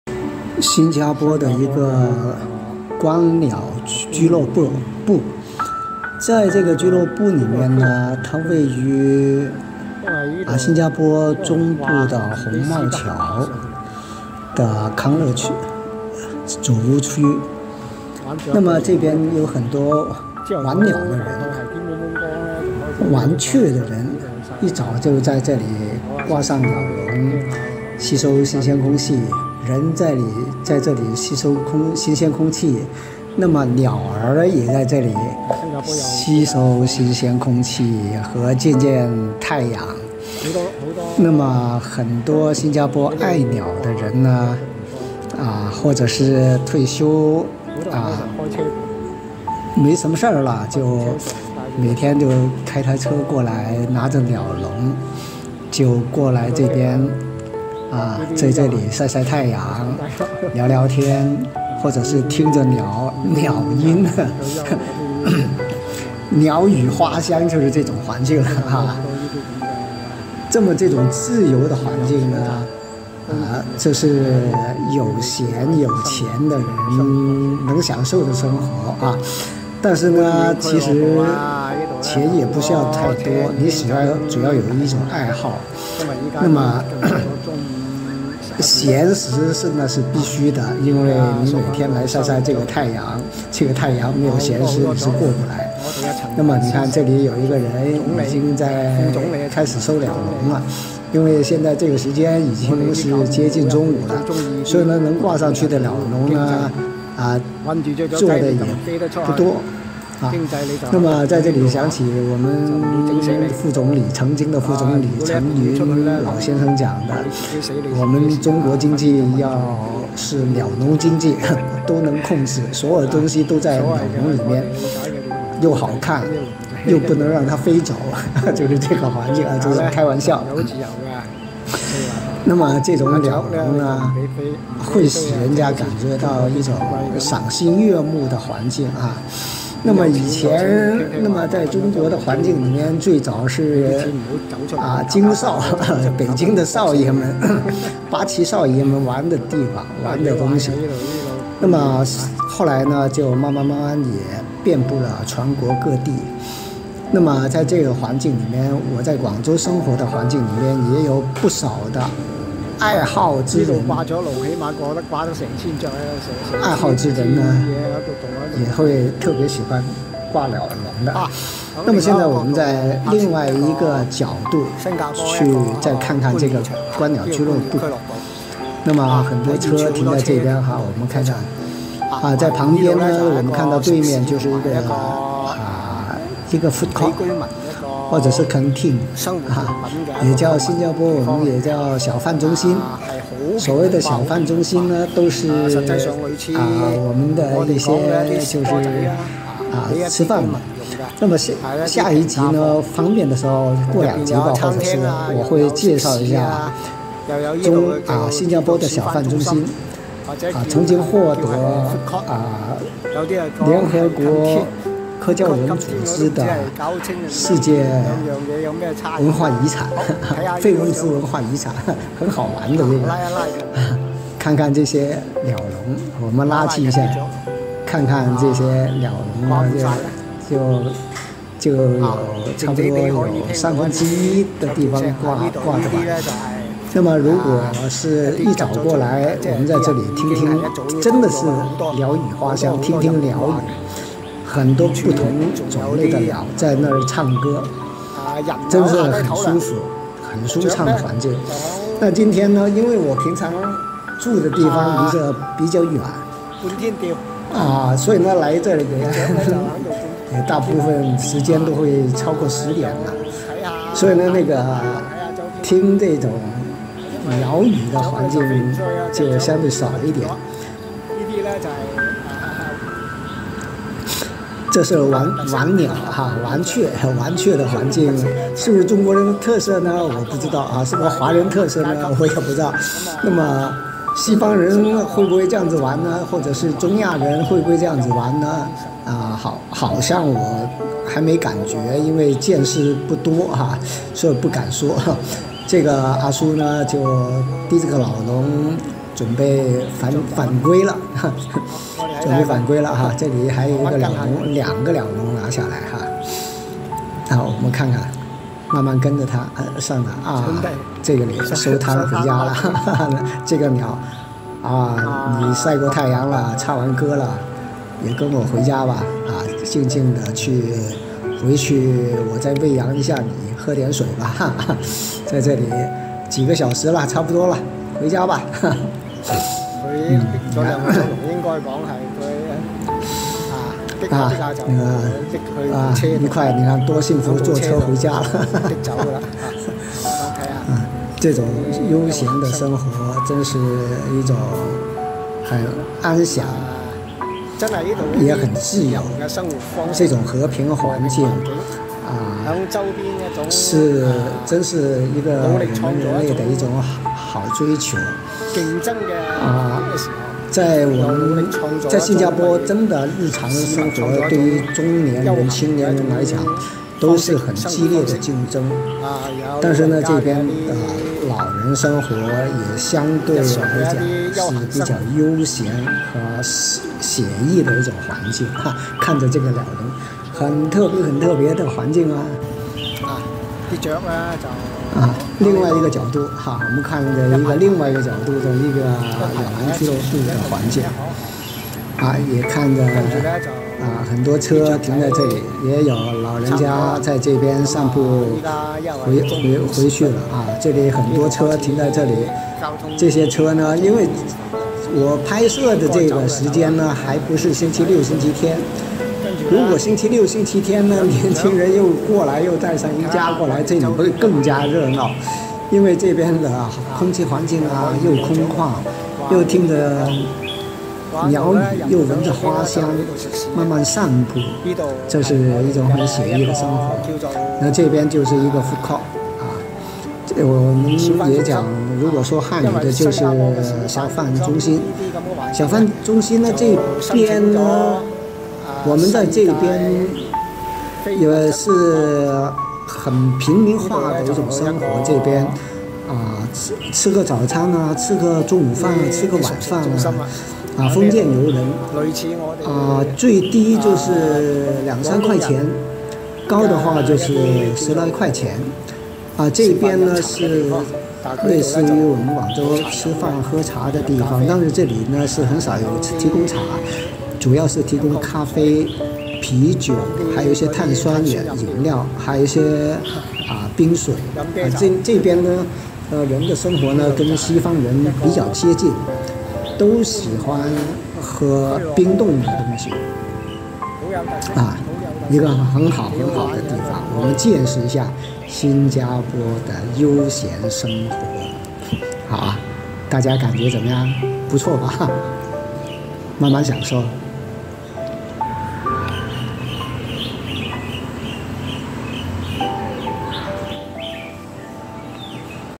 20230119新加坡玩鳥人愉悅地--宏茂橋聽百鳥鳴之“晾鳥”
”鳥樂園”位於宏茂橋永康園組屋區內，真是個有趣又獨特的地方，值得一去。宏茂橋觀鳥俱樂部是新加坡最大的、歷史最悠久的鳥類聚集場所，每天都有眾多鳥類愛好者在這裏遛鳥炫鳥。